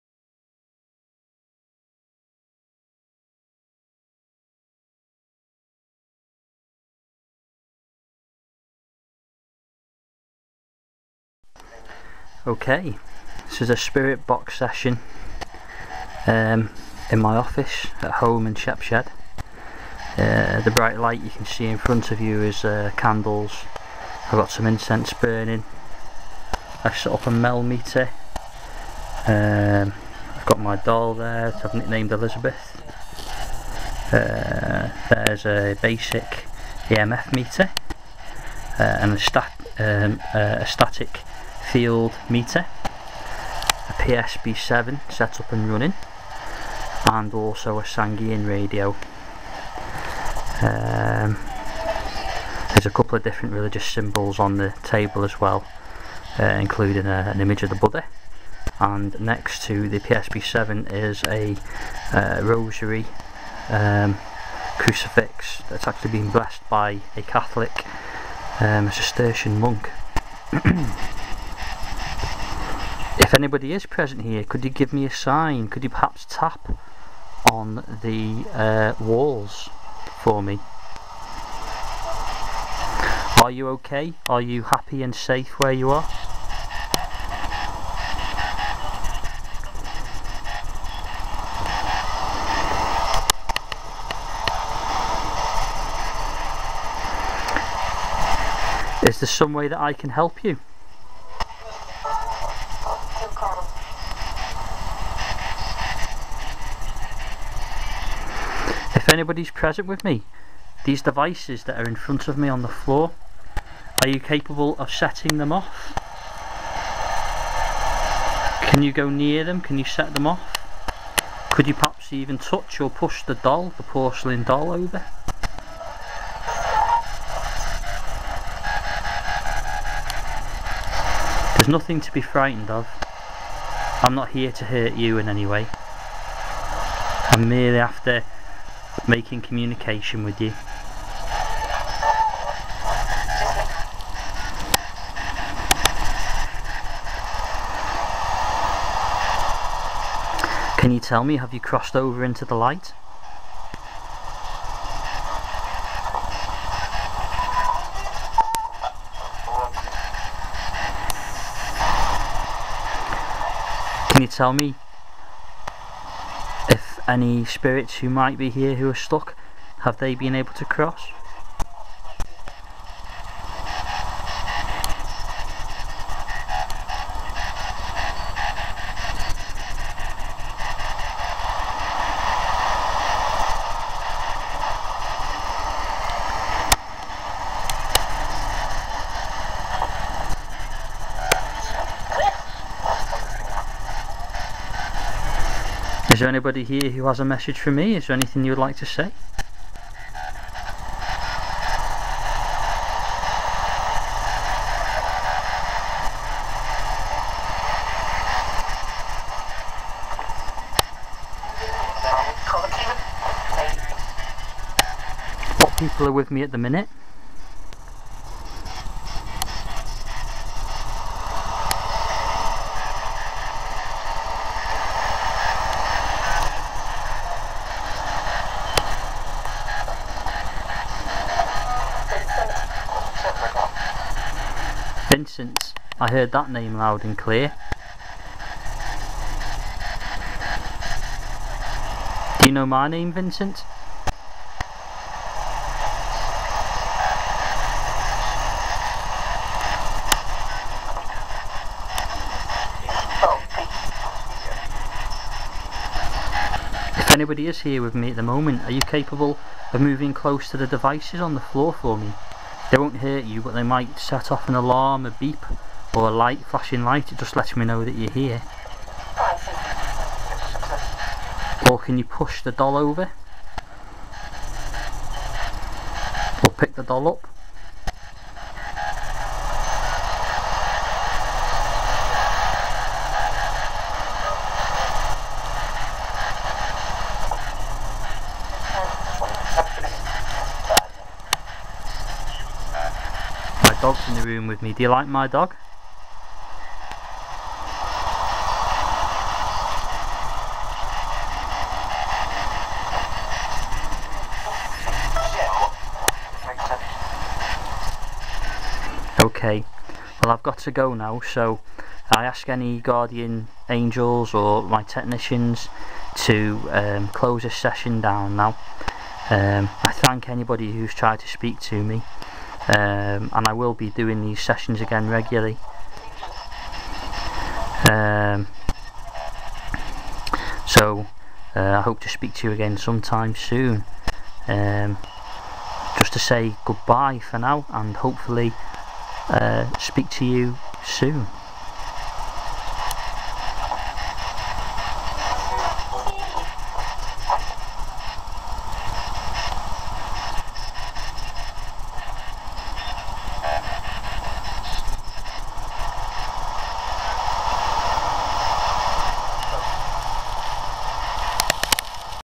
Ghost Box Session 17th January 2013
Clips from a Ghost Box Session in my office at home. There was quite a bit of quiet EVP on here including quite a cool one that says "She hurts us", I've stuck to the clearest loudest clips for the sake of this video.